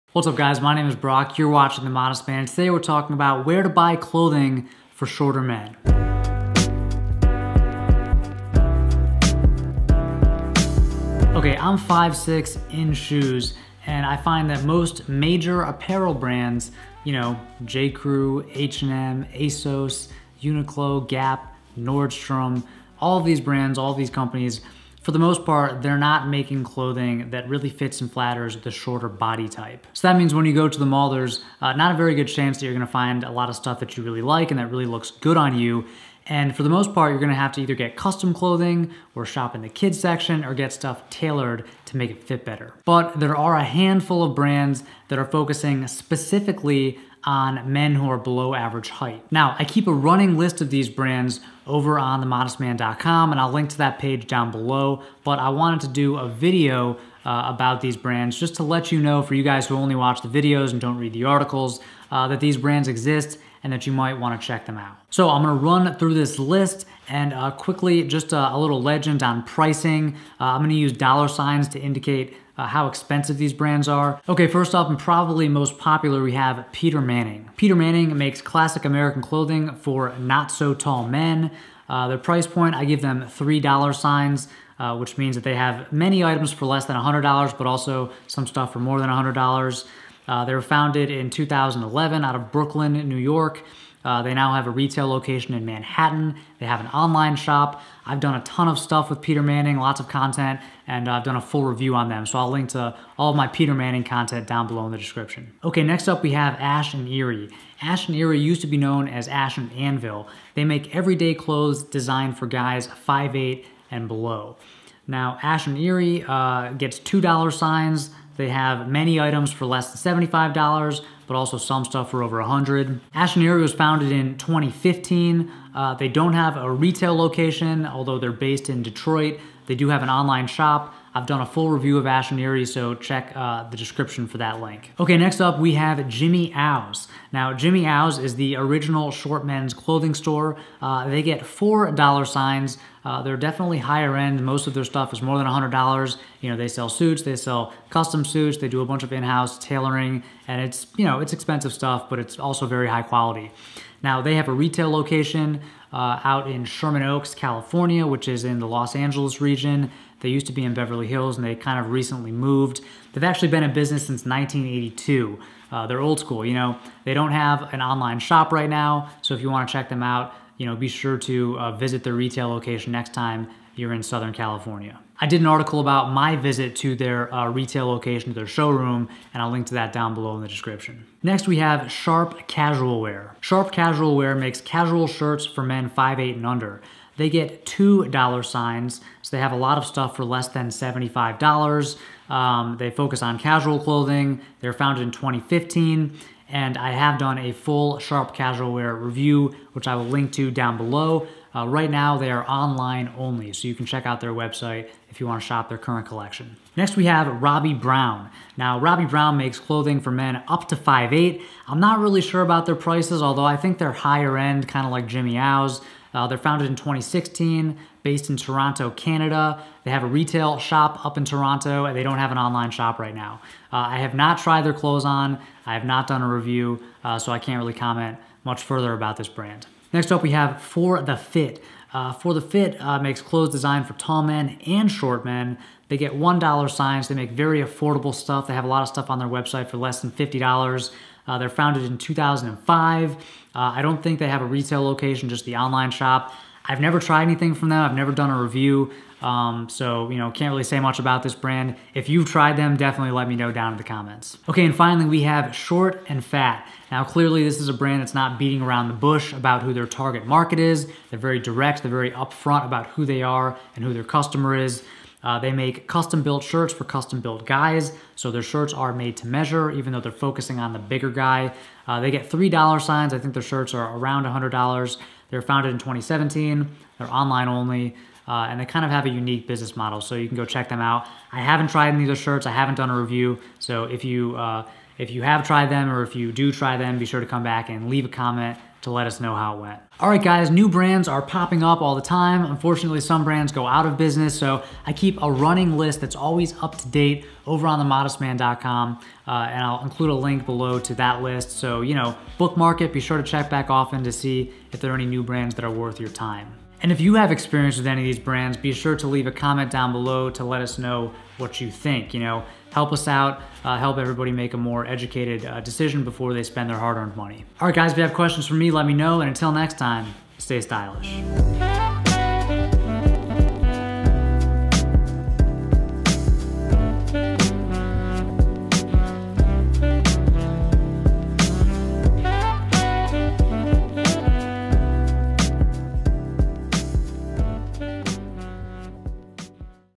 Where to Buy Clothes for Short Men | Complete List of Brands
Most clothing brands completely ignore short men. They make clothes for men of average height and taller men. But there are some brands who focus exclusively on shorter men. If you haven't tried these speciality brands, I highly recommend giving them a shot.

Some of them have actual brick and mortar stores, some operate online, and some do both. They all have different styles and price points, from super affordable to very expensive.

MY GEAR
▪ Canon T6i
▪ Lens #1 (50mm)
▪ Lens #2 (10-18mm)
▪ Lens #3 (24mm)
▪ Lav mic
▪ Desk mic